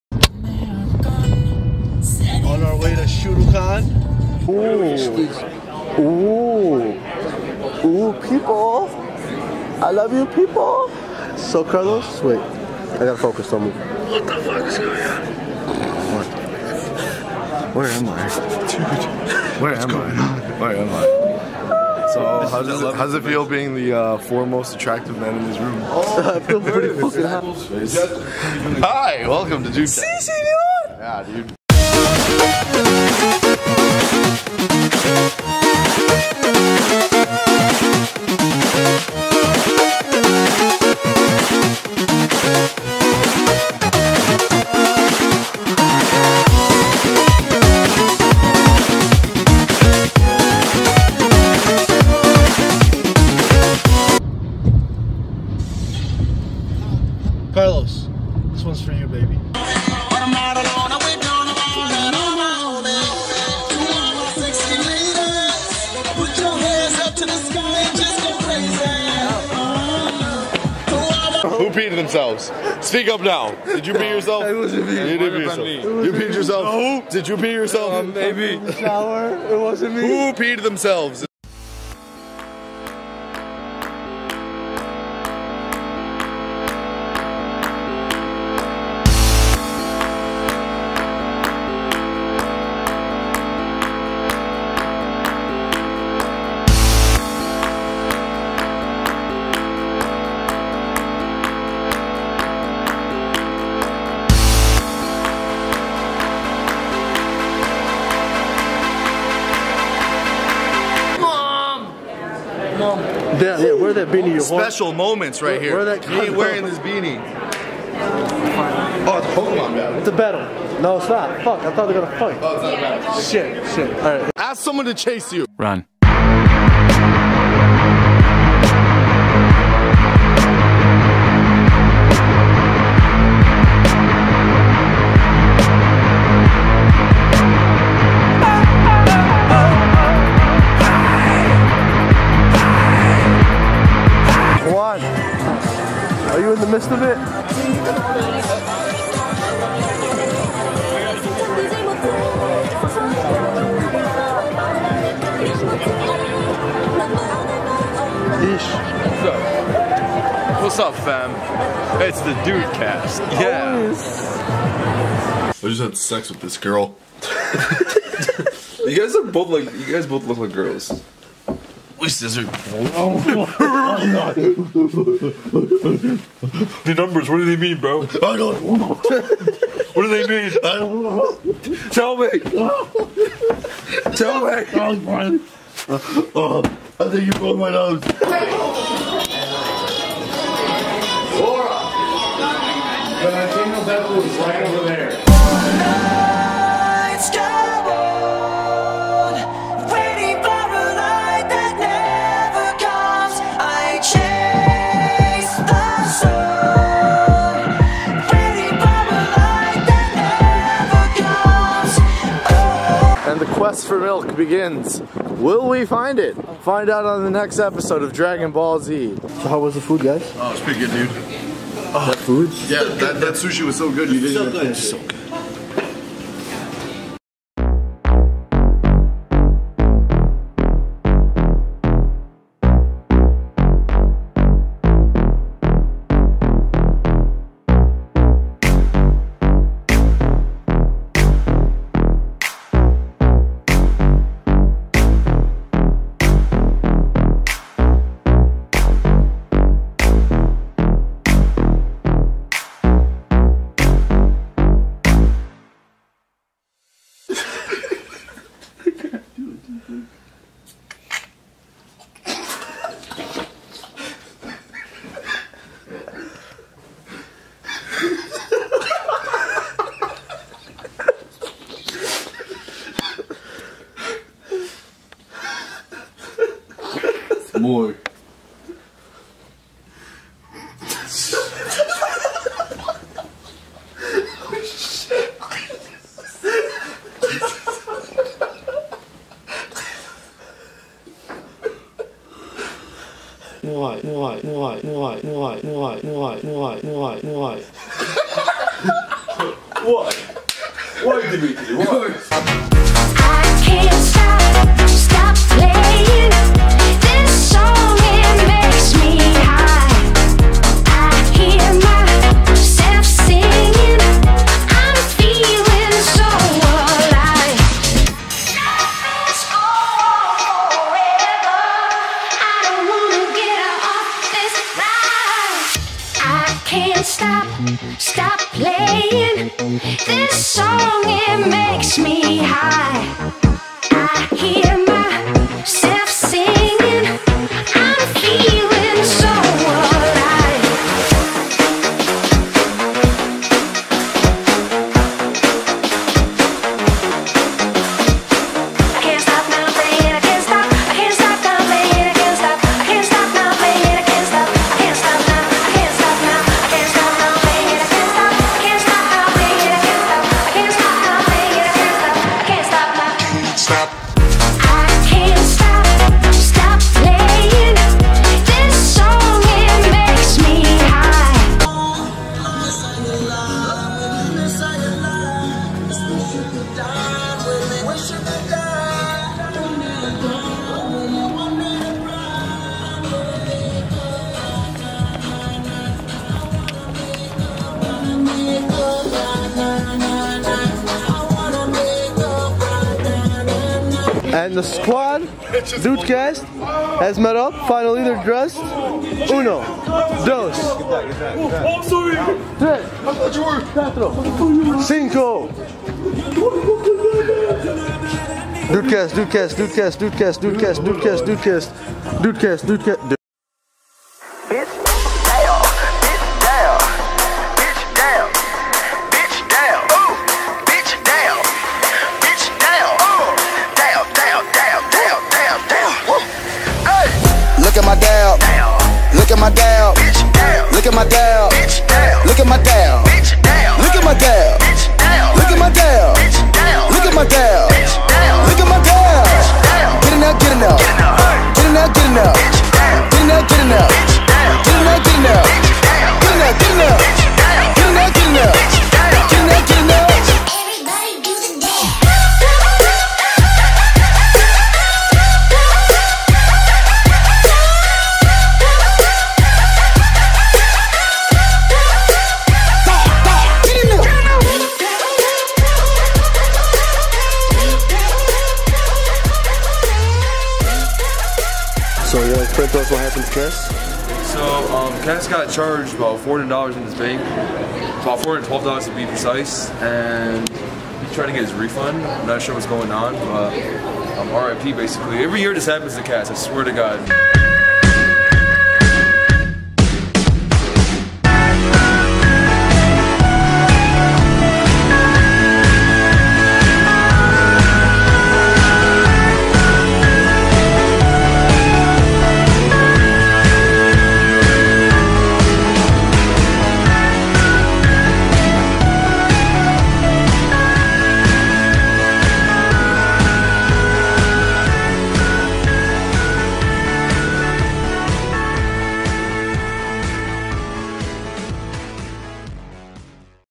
SHUTOCON 2016 VLOG!
The D00D's explore Shuto Con 2016!